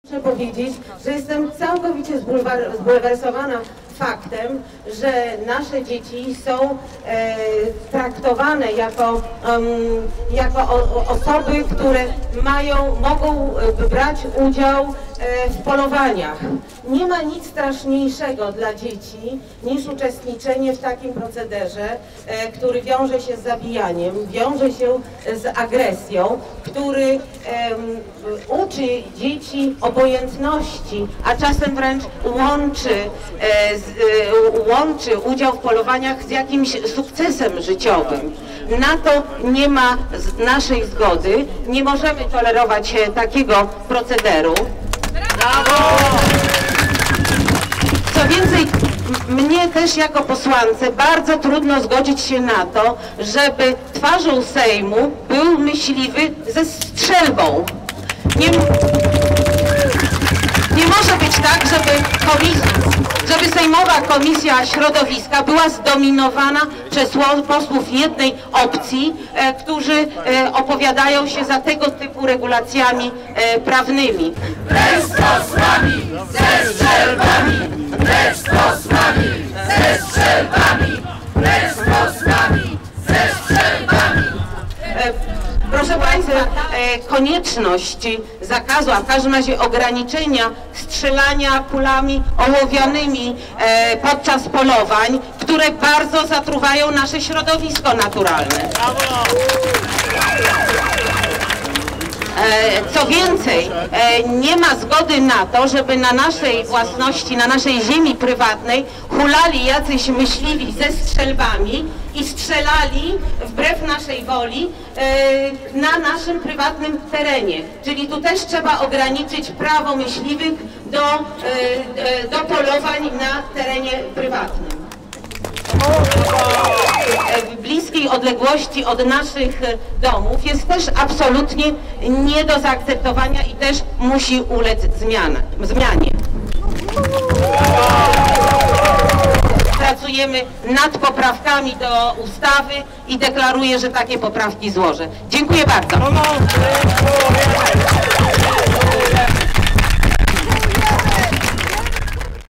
Wicemarszałkini Sejmu RP Wanda Nowicka o polowaniach z udziałem dzieci
Wicemarszałkini Sejmu RP Pani Wanda Nowicka wypowiada się przeciwko udziałowi dzieci w polowaniach podczas manifestacji "Dość rządów lobby myśliwskiego w naszym kraju!" zorganizowanej przez koalicję Niech Żyją! skupiającą kilkadziesiąt organizacji przyrodniczych i prozwierzęcych.
Warszawa, 26 maja 2015 r.